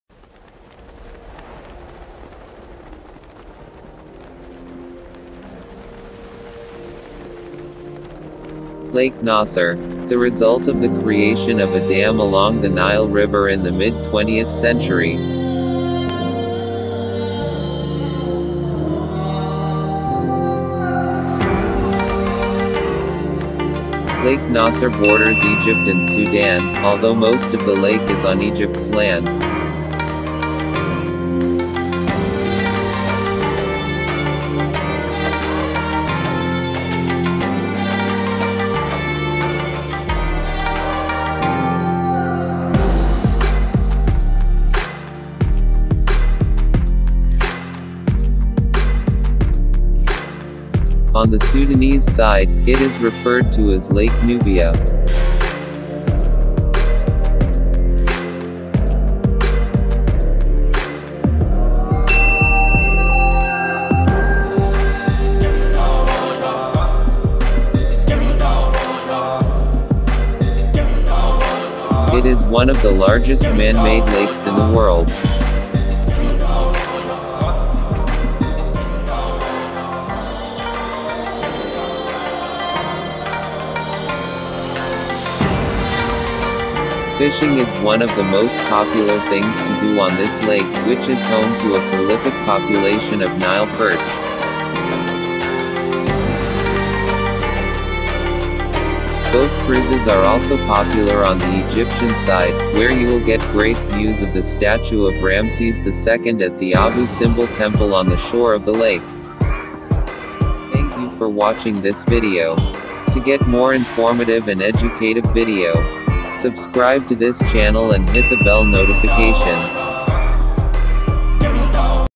The biggest Man-made Lake - Lake Nasser, Egypt
Lake Nasser, Egypt is one of the biggest Man-made Lake in the world. It is a sight to behold.